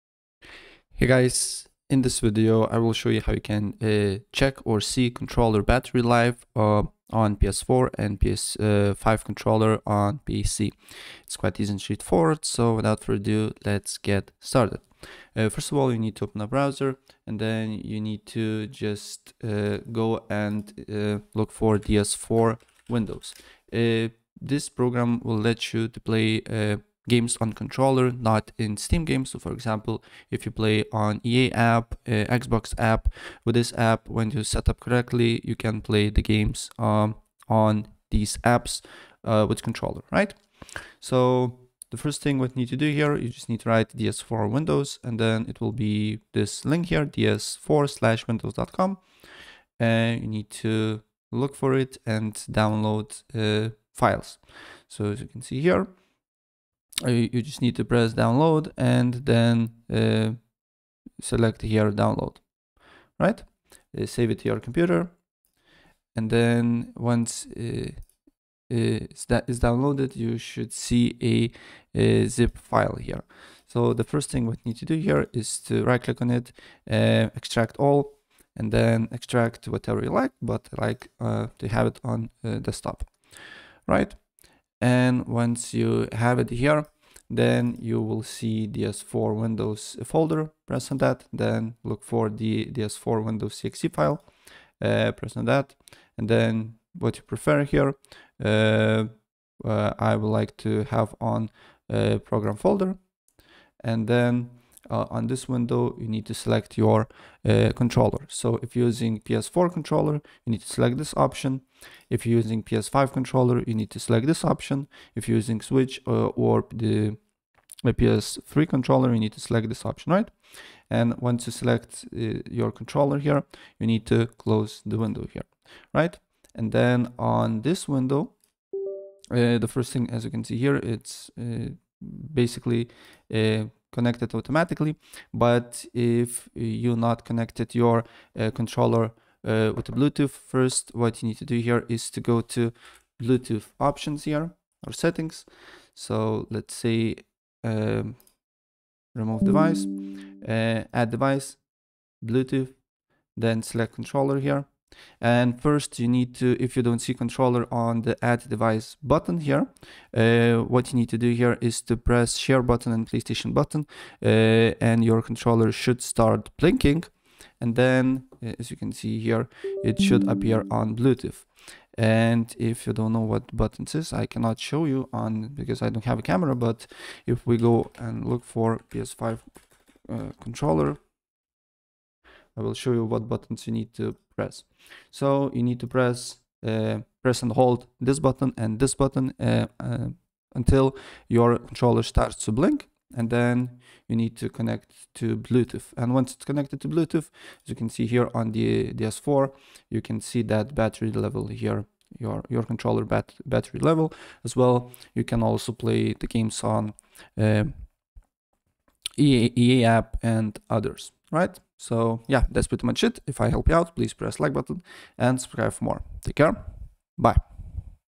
How To Check PS4 / PS5 Controller Battery Life On PC - Full Guide 2025
In this video I will show you How To Check PS4 PS5 Controller Battery Life On PC  It's really easy and it will take you less than a minute to do it!